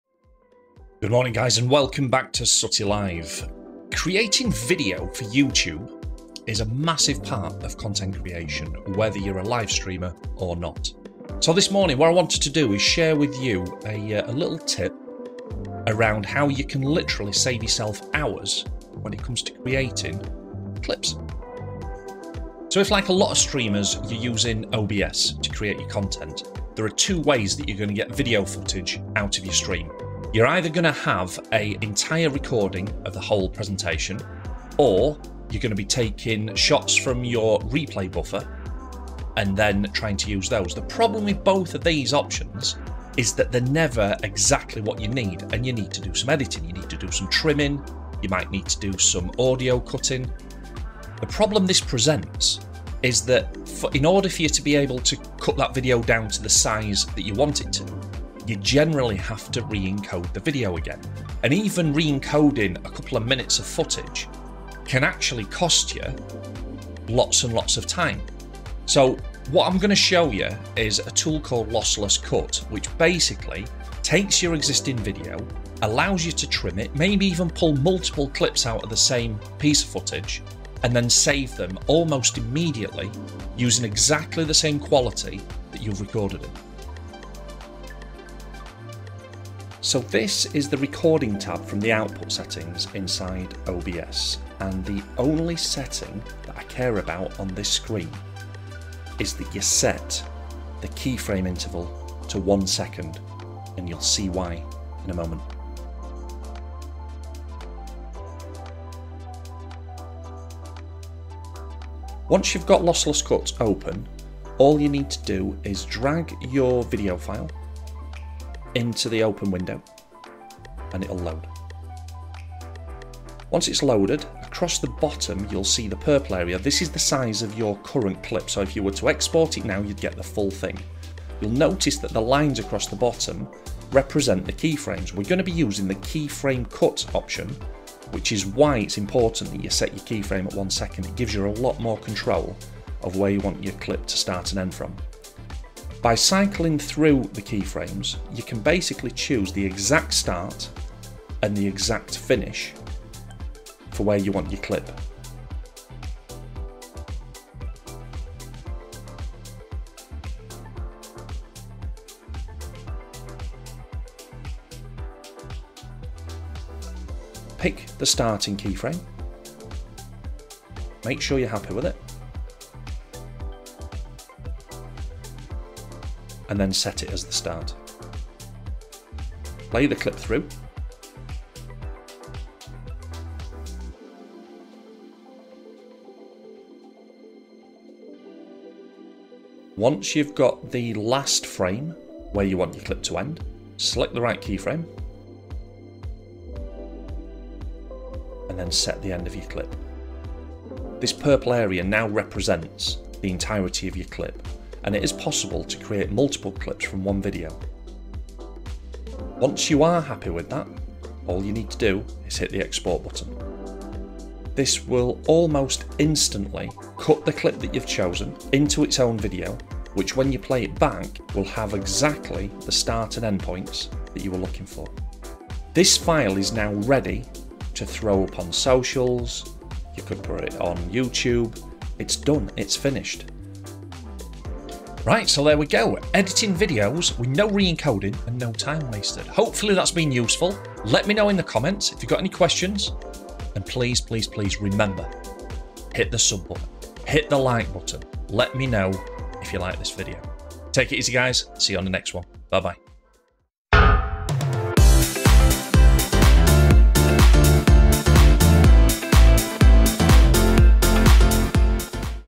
Edit Video Clips Quickly Without Re Encoding
If you want to Edit Video Clips Quickly Without Re Encoding, you need to take a look at this tutorial.  I cover a simple setting in OBS Studio along with an essential video editing tool that's going to save you time.